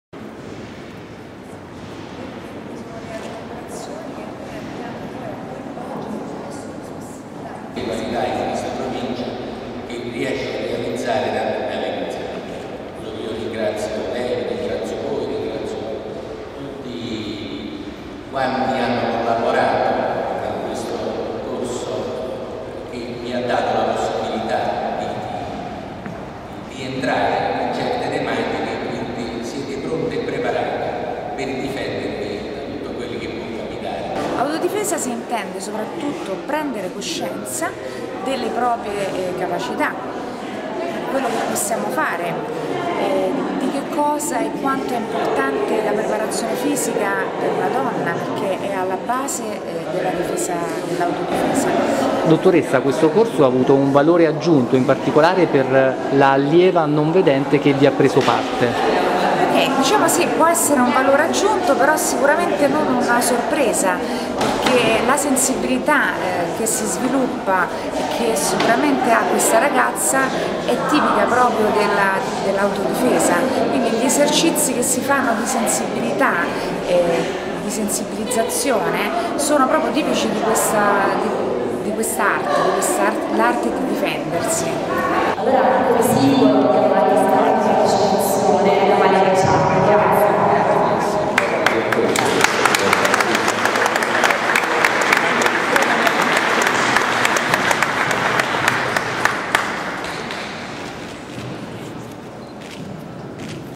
Corso di autodifesa promosso dalla Provincia
Venerdì 25 novembre si è svolta, presso il Palazzetto dello Sport di Caserta, la cerimonia di conclusione del corso di autodifesa per donne promosso dalla Provincia di Caserta attraverso l' "Ufficio della Consigliera provinciale di Parità" in collaborazione con l' "Agis- Agenzia per la Gestione degli Impianti Sportivi della Provincia di Caserta". Un' esperienza utile per le allieve che vi hanno preso parte che nella serata di venerdì hanno tenuto una simulazione sulle techiche apprese durante il corso, coordinate da Claudio Parretta, istruttore di difesa personale Mga 3° livello  e cintura nera di karate 5° dan, accompagnato per l'occasione da Cinzia Colaiacomo, componente della commissione tecnica federale Mga e direttore tecnico del gruppo sportivo karate Fiamme Oro della Polizia dello Stato.

Presenti all'appuntamento, tra gli altri, il presidente Domenico Zinzi e la consigliera provinciale di parità Francesca Sapone, tra i principali promotori dell'iniziativa, soddisfatti per il successo ottenuto dal corso di autodifesa, un'occasione per le circa 40 partecipanti per imparare divertendosi riuscendo anche a socializzare.